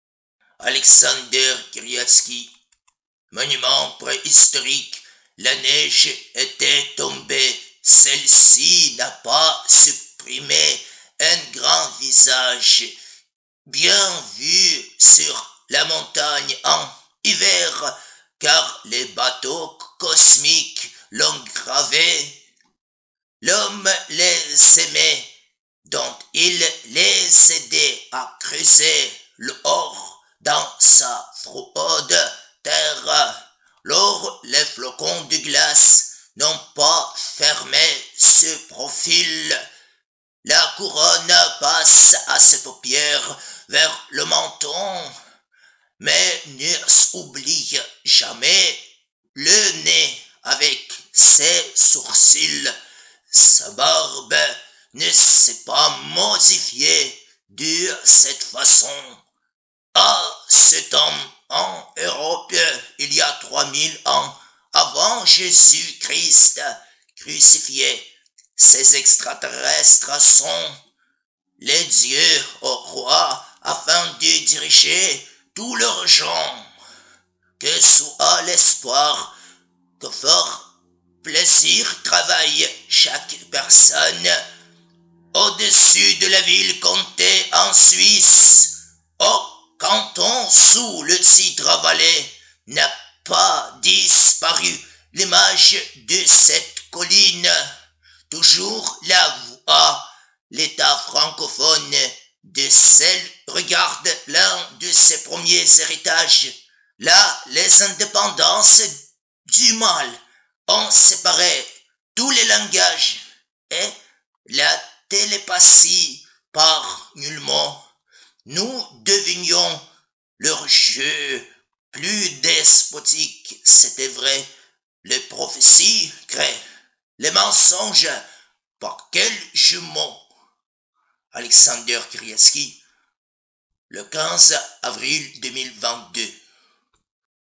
Monument préhistorique / Alexander Kiriyatskiy - ÉG.
ÉGALISEUR . Le texte du vers se trouve sous le plus, en bas.
Alexander Kiriyatskiy:
Monument préhistorique (Haut de Cry)

La neige était tombée, celle-ci n’a pas supprimé
Un grand visage bien vu sur la montagne en hiver,
Car les bateaux cosmiques l’ont gravé, l’homme les aimait.
Dont il les aidait à creuser l’or dans sa froide Terre.

Lors les flocons de glace n’ont pas fermé ce profil,
La couronne passe à ses paupières, vers le menton.
Mais ne s'oublient jamais le nez avec ses sourcils
Sa barbe ne s’est pas modifiée de cette façon.

À ce temps en Europe, il y a trois mille ans avant
Jésus Christ crucifié, ses extraterrestres sont
Les dieux aux rois afin de diriger tous leurs gens
Que soit l’espoir, qu’aux forts plaisirs travaille chaque personne.

Au-dessus de la ville Conthey en Suisse au canton
Sous le titre Valais n’a pas disparu l’image
De cette colline. Toujours la voit l’état francophone.
De celle, regarde l’un de ses premiers héritages.

Là, les indépendances du mal ont séparé
Tous les langages et la télépathie par nul mot.
Nous devenions leurs jeux plus despotiques, c’était vrai.
Les prophéties créent les mensonges par quels jumeaux.